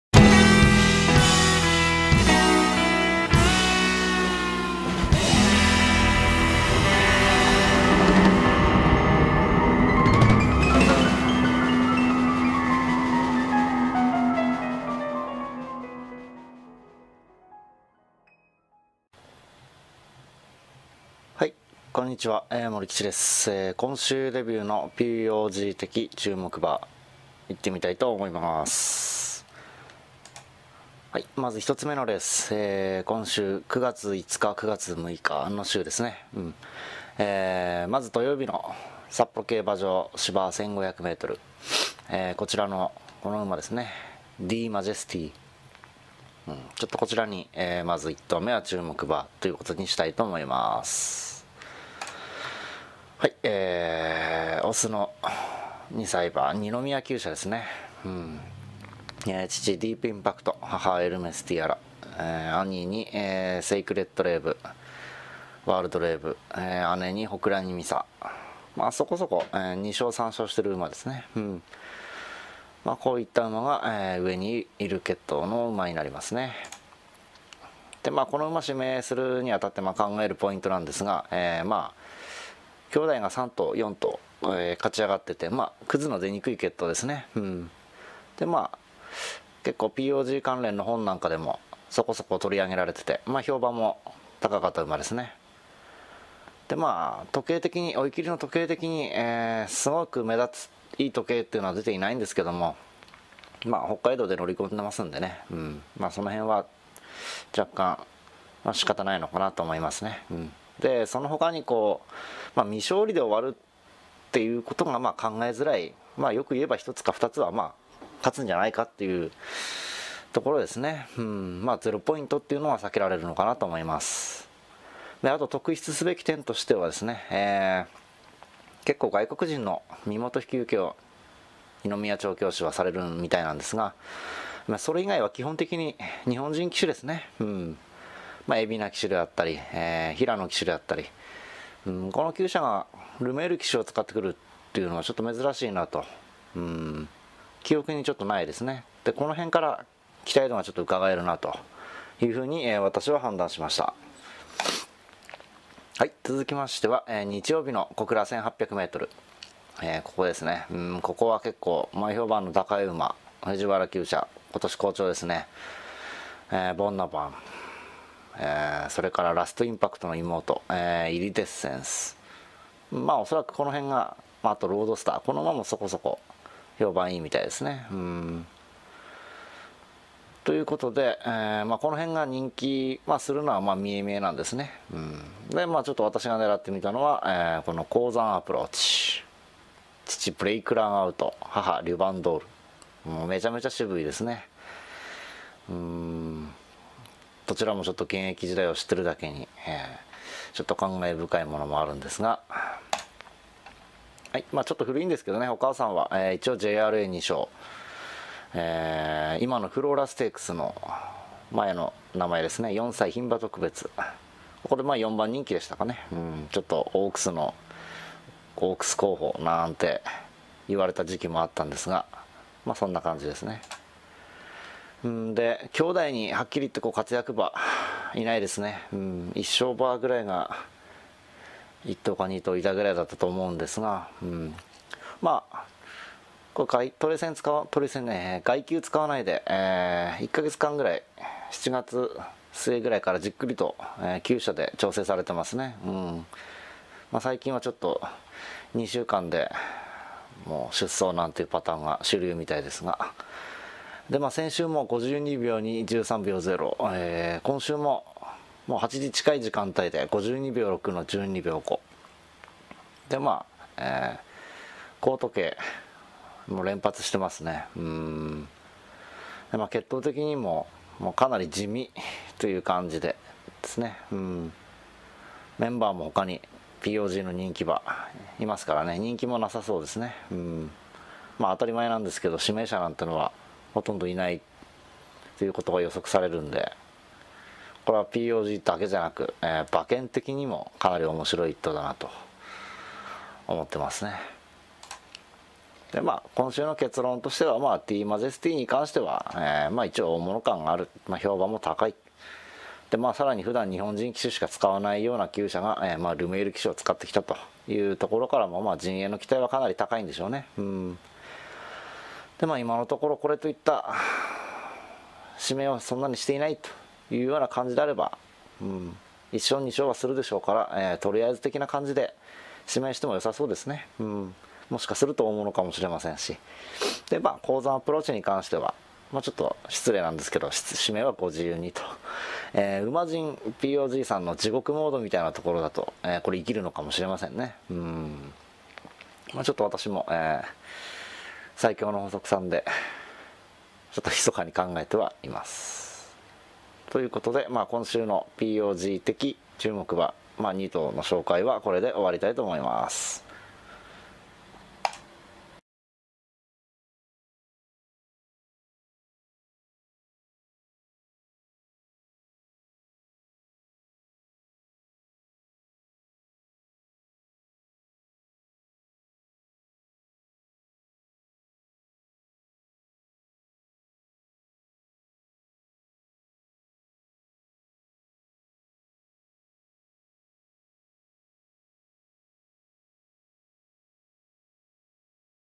【競馬】POG 今週はディーマジェスティと誰も注目していないあの馬に注目！
list=PLF0J5bzPL78ZYG34AyuJdE5BUNnB58w7Y

面白い奴だなと思っていただけましたら、